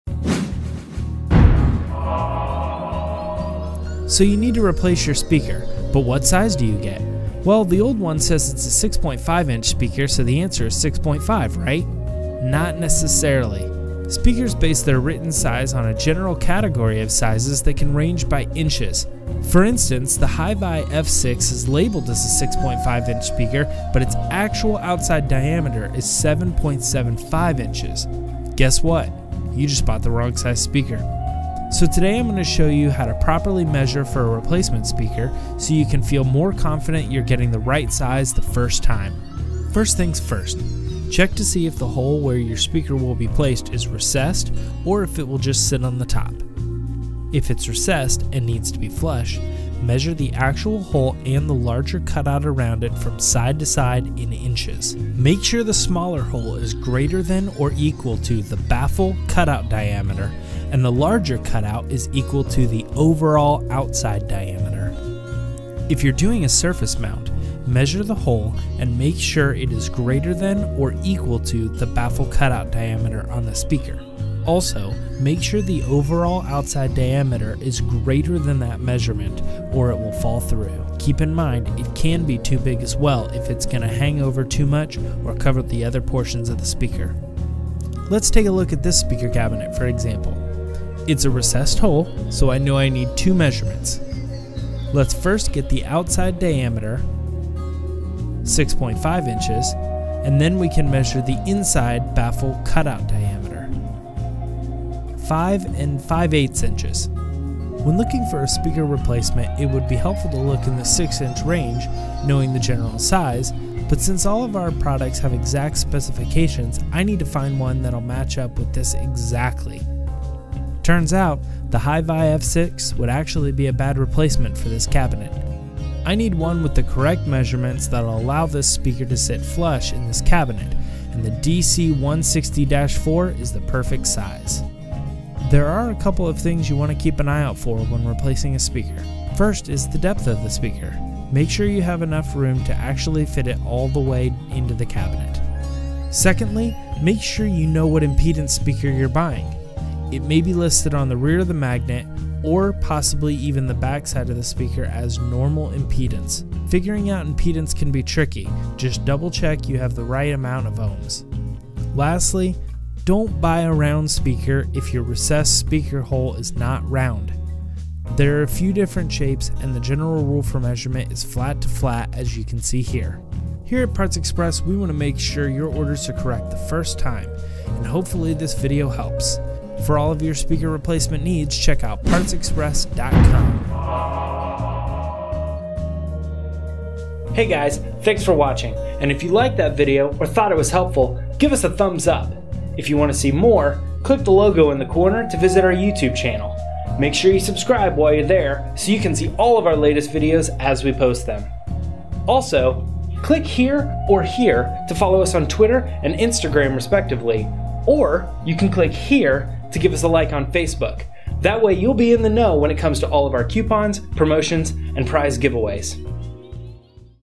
How to Measure for a Replacement Speaker.
DESCRIPTION:
So you just blew a speaker or are gonna be upgrading soon but you're not sure what size speaker your gonna need. There are a few things to keep in mind before you assume you know what size to buy and this video will show you what to look for.

Don't put yourself in that position, trying to stick your brand new speaker that just arrived into your enclosure only to find out, IT DOESN'T FIT?!?!

Watch this video first! :)

*There are many factors that go into choosing the right speaker for a replacement or upgrade. This video is only to meant to be a guide on measuring for speaker size.